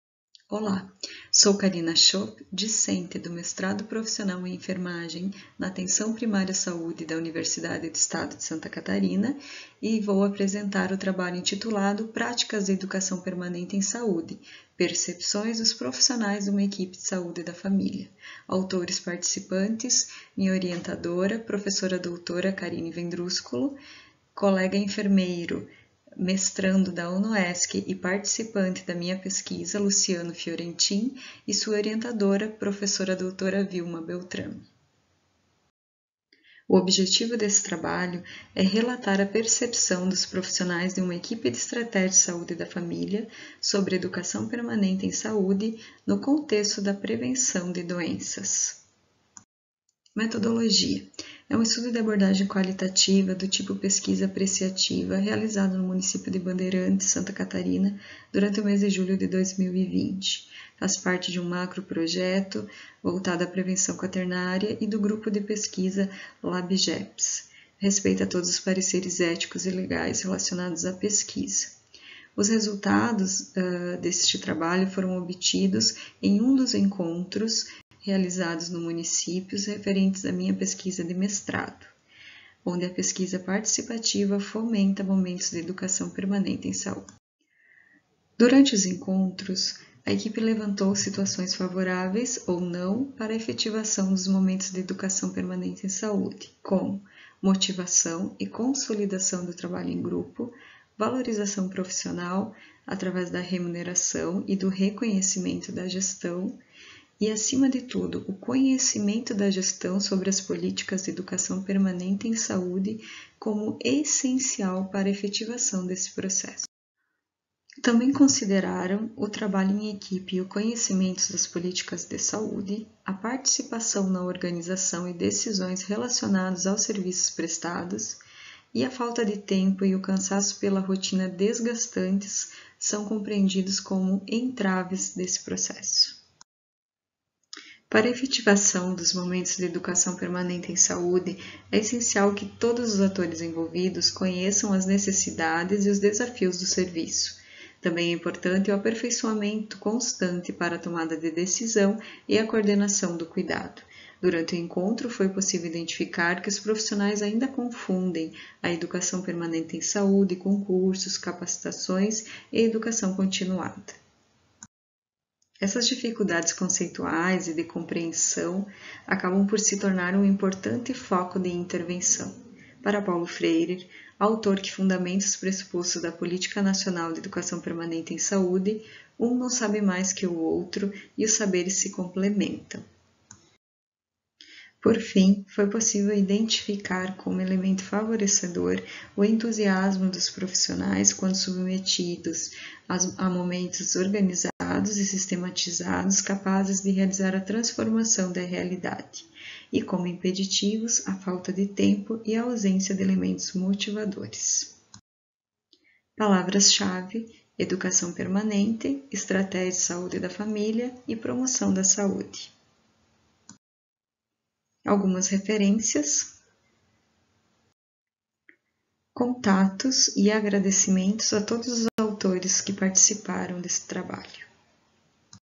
Prática de Educação Permanente em Saúde: percepções dos profissionais de uma equipe de Saúde
Trata-se de um relato da percepção dos profissionais de uma equipe da Estratégia Saúde da Família (eSF) sobre Educação Permanente em Saúde no contexto da prevenção de doenças. Como parte de um estudo de abordagem qualitativa do tipo Pesquisa Apreciativa. Onde a metodologia participativa fomenta a EPS e, nessa direção, evidenciou-se percepções dos profissionais sobre este tema.